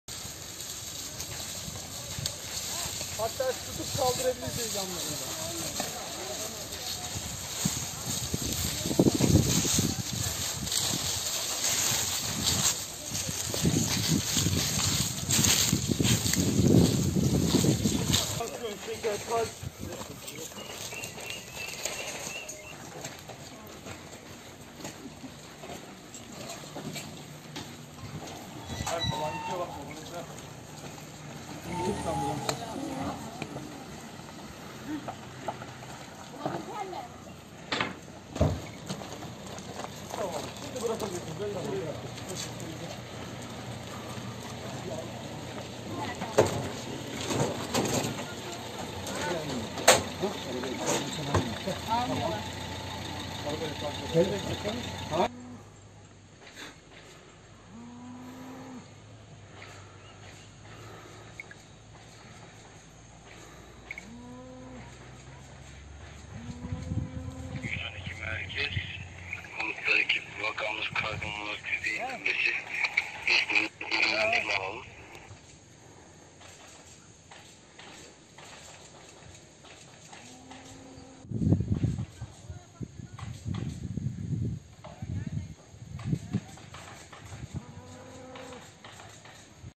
DUMANLARIN ARASINDA KALDI
Düzce’de, evinin arkasındaki fındık bahçesinde anız yakarken kuru otların tutuşması sonrası dumanların arasında kalan yaşlı adam zehirlendi.